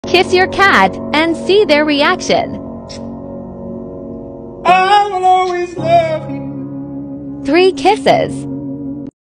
MY CAT IS BROKEN 😭
My cat is broken! Normally when I try to kiss him he bites me or plays rough! I got 3 kisses in!